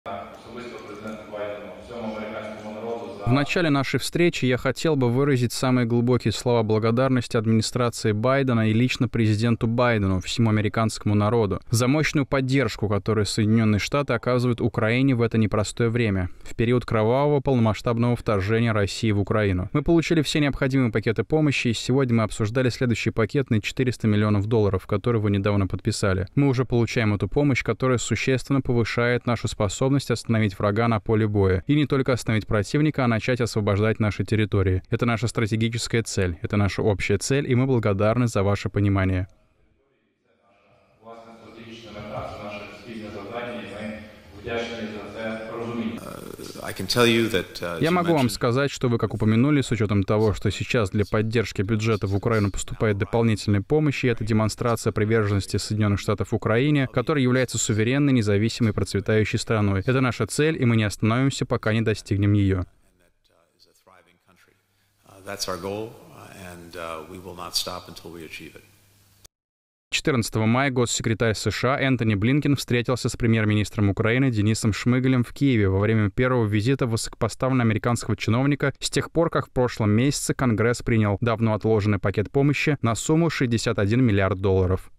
США не отступят от Украины: Вашингтон обещает поддержку Киеву
Госсекретарь США Энтони Блинкен встретился с премьер-министром Украины Денисом Шмягалем в Киеве во вторник в рамках первого визита высокопоставленного американского чиновника с тех пор, как в прошлом месяце Конгресс принял давно отложенный пакет помощи на сумму 61 ​​миллиард долларов.